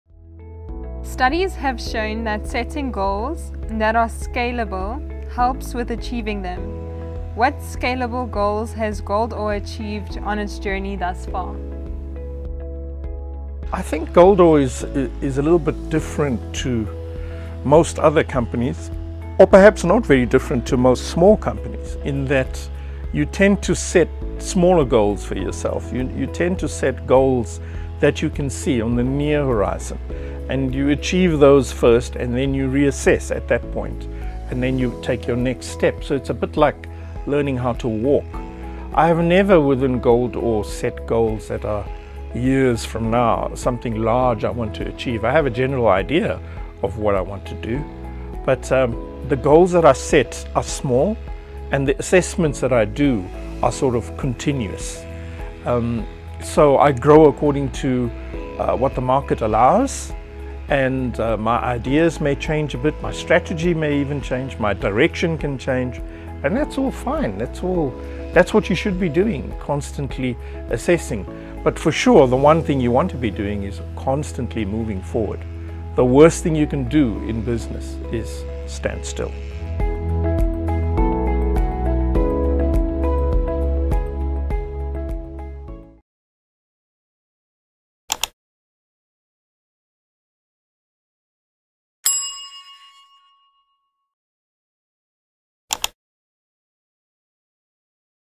What scalable goals has GoldOre achieved?
Studies have shown that setting goals that are scalable help with achieving them. What scalable goals has GoldOre (Pty) Ltd. achieved on its journey thus far?

GoldOre works a bit differently compared to other internationally successful companies. In a way, it works more similarly to some small-scale companies in the way that GoldOre sets very small goals. These goals are very visible, seen just on the horizon, and are able to be achieved in the near future.

Once these small goals are achieved, you are able to step back and reassess. Then, you’re able to set the next tangible goal. This process can even be likened to learning how to walk, taking one small step at a time.

Although GoldOre is a company with a set idea of where it would like to be in the future, we do not fully believe in setting specific goals years in advance. Instead, GoldOre focuses on setting and completing small goals, one at a time.

This method of goal setting allows for changes or readjustments that are inevitable along the journey of growth within a business. In an ever-changing world, you have to learn to continually assess the business environment and adjust your strategy accordingly. A kind of dynamic and continuous subconscious SWOT analysis.

The one thing that all of GoldOre’s goals have in common, they work towards forward movement, growth, and improvement. The worst thing a business can do is stand still. Every day gets our full attention, and every day is a new opportunity for us to grow and innovate, without worrying too much about the past and the future.

We prefer truly being in the present moment and making the most out of every opportunity.